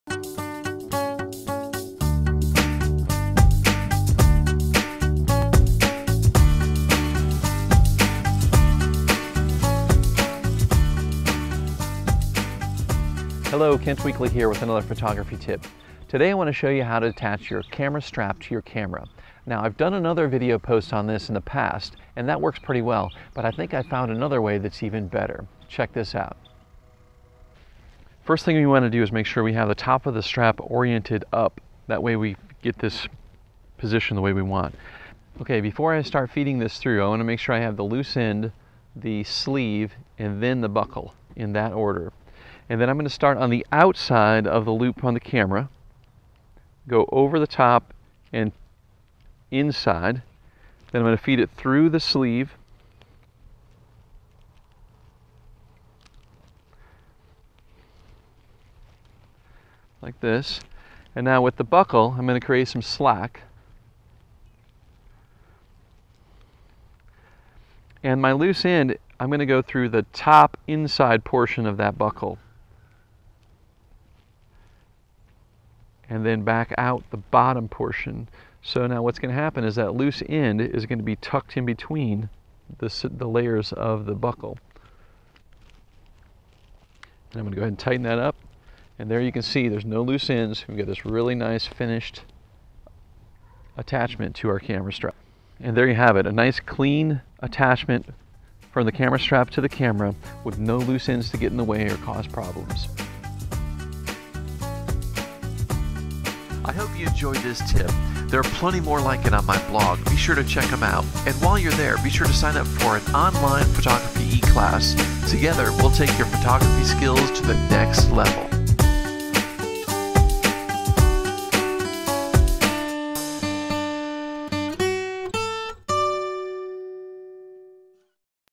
Photo Tip ~ Best Way To Attach Camera Strap
Best Way To Attach Camera Strap. No loose ends. Nice finished factory look. Best way for any photographer to photograph -- no loose ends.